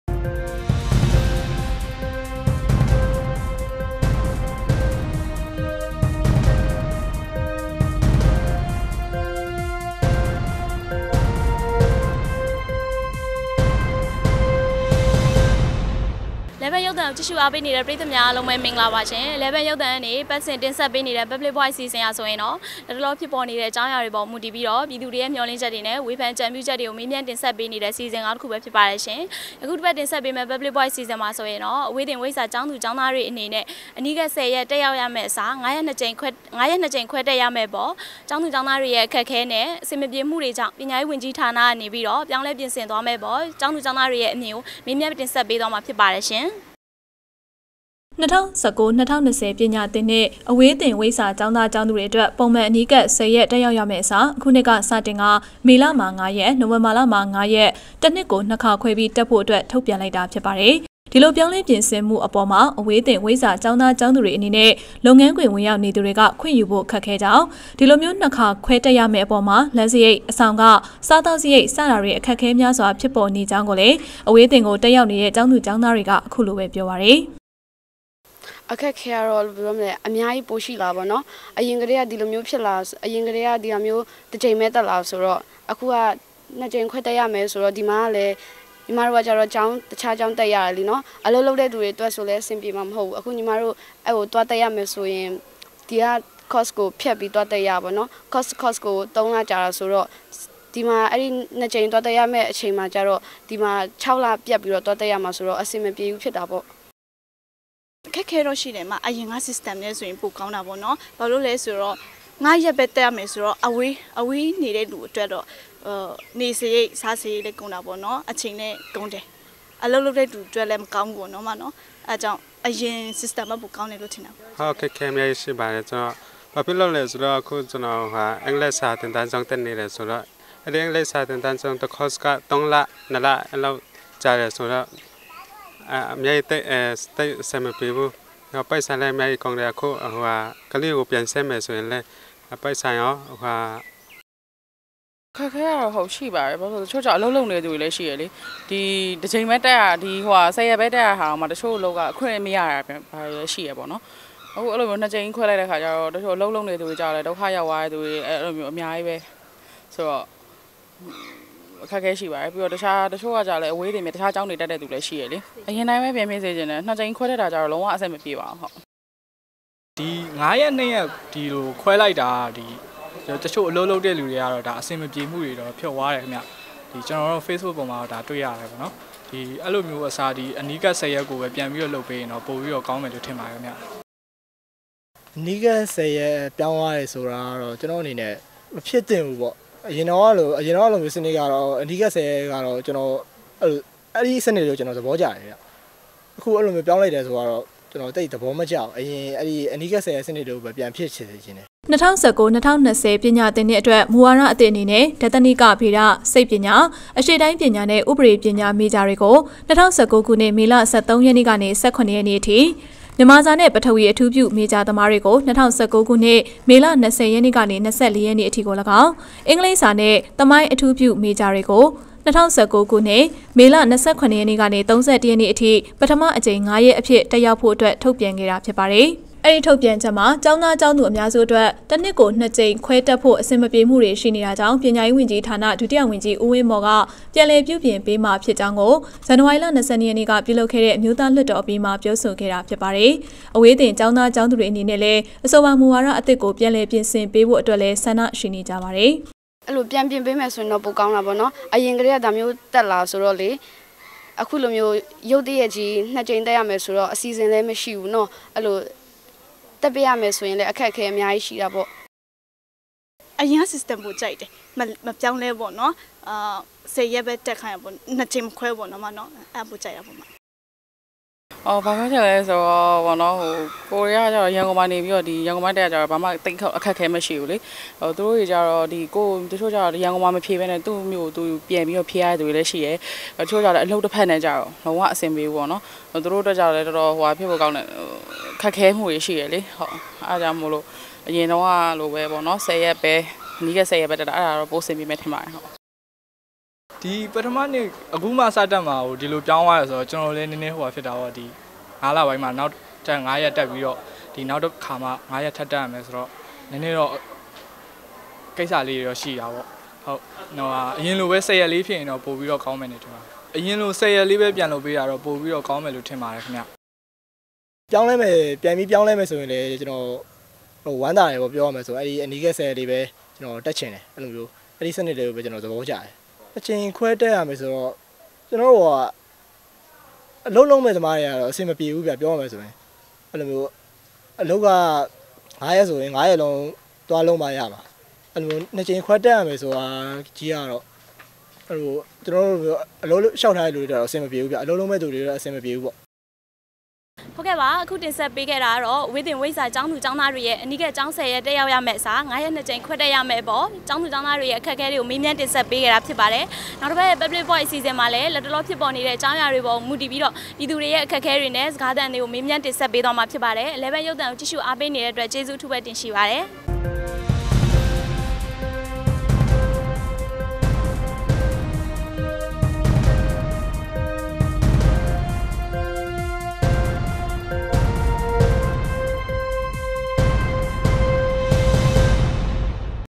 ၂၀၁၉- ၂၀၂၀ ပညာသင္ႏွစ္ အေ၀းသင္( ၀ိဇၨာ)ေက်ာင္းသား ေက်ာင္းသူမ်ား
၂၀၁၉- ၂၀၂၀ ပညာသင္ႏွစ္ အေ၀းသင္( ၀ိဇၨာ)ေက်ာင္းသား ေက်ာင္းသူမ်ားအတြက္ ပံုမွန္ အနီးကပ္ ၁၀ရက္တက္ေရာက္ရမည့္အစား ယခုႏွစ္မွစတင္ကာ ေမလတြင္ ၅ရက္၊ ႏို၀င္ဘာလတြင္ ၅ရက္ တစ္နွစ္လွ်င္ႏွစ္ခါခြဲျပီးတက္ရန္အတြက္ ထုတ္ျပန္လိုက္တာျဖစ္ပါတယ္။